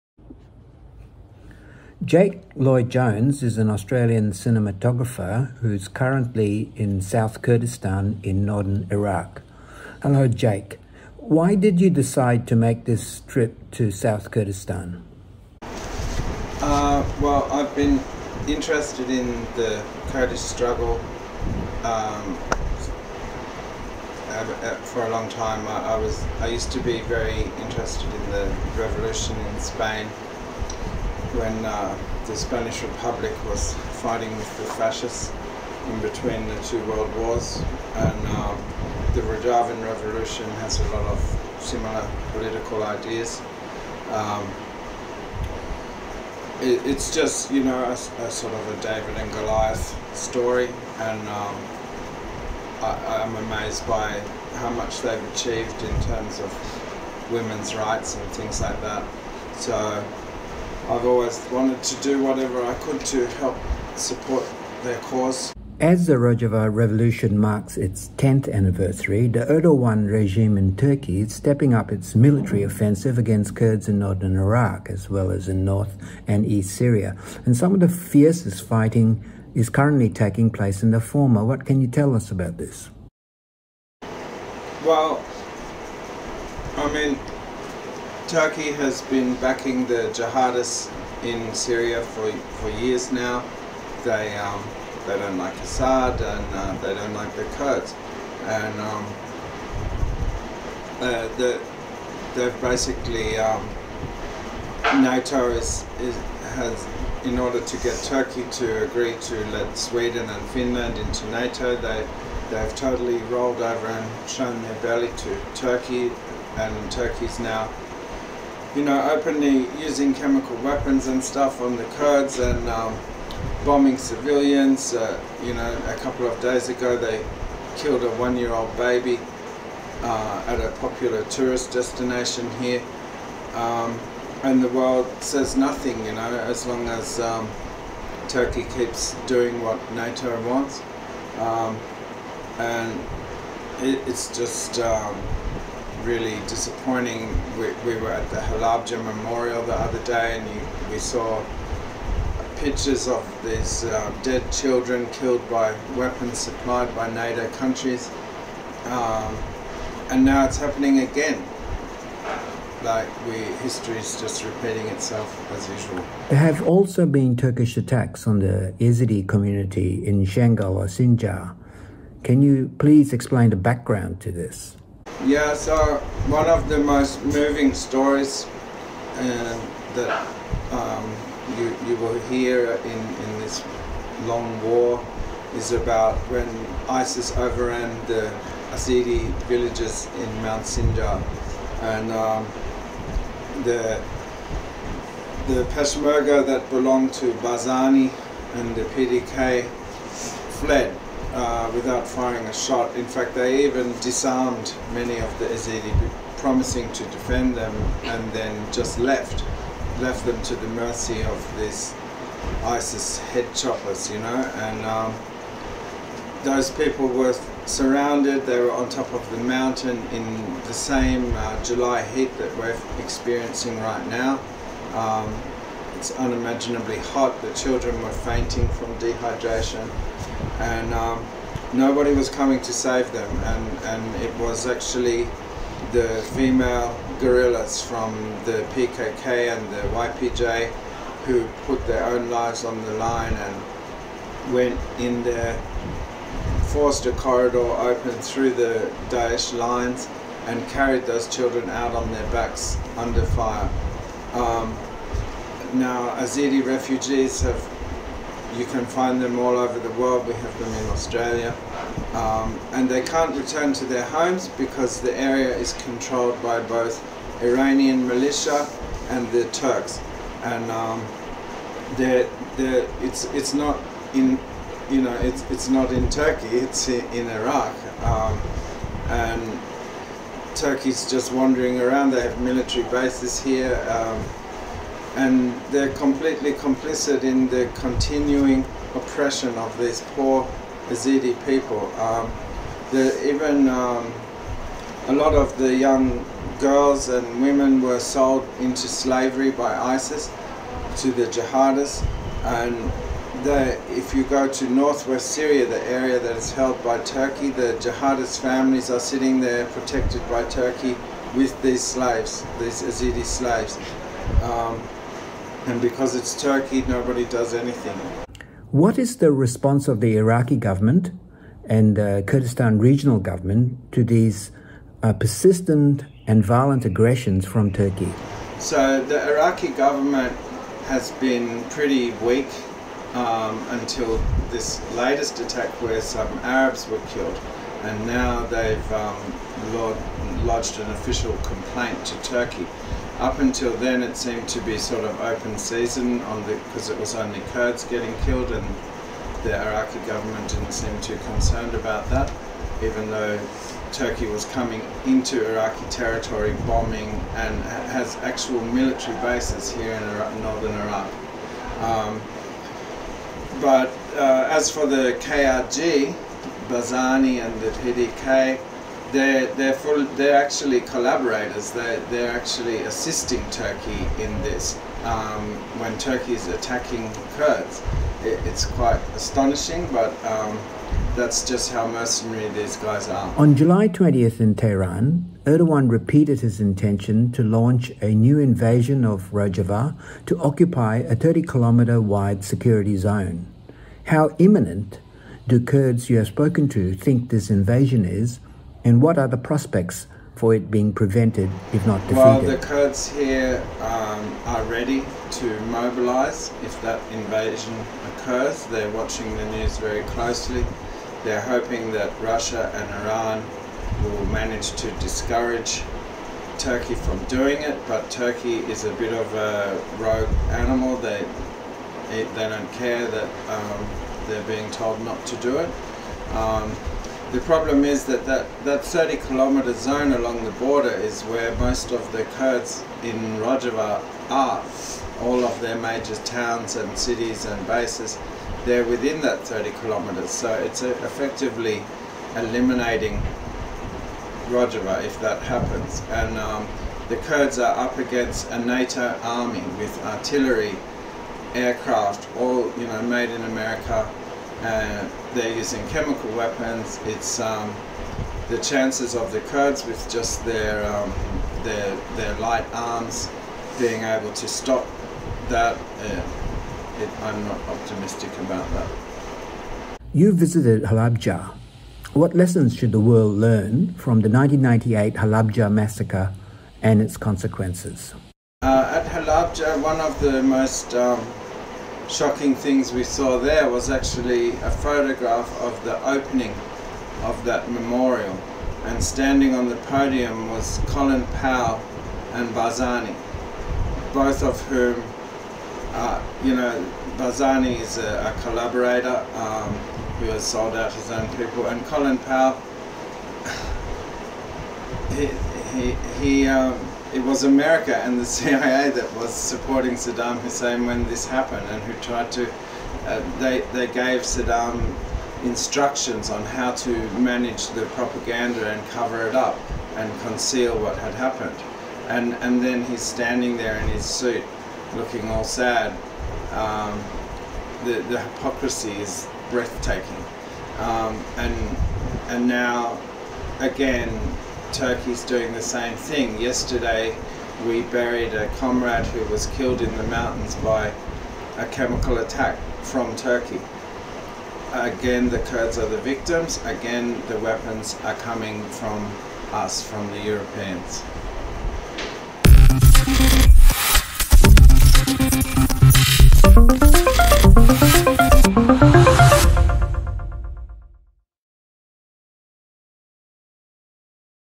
'A David and Goliath struggle' — eyewitness report from South Kurdistan
Australian cinematographer Jake Lloyd Jones is in Bashur (South Kurdistan in northern Iraq) observing first hand the ongoing resistance of the  Kurds to the Turkish state's escalating war against them. He spoke to Green Left's Peter Boyle on July 22 about this "David and Goliath struggle" just two days after Turkish military shelled a popular tourist spot in the Iraqi province of Duhak, killing a least nine people (including) children). Video footage courtesy of MerJa Media.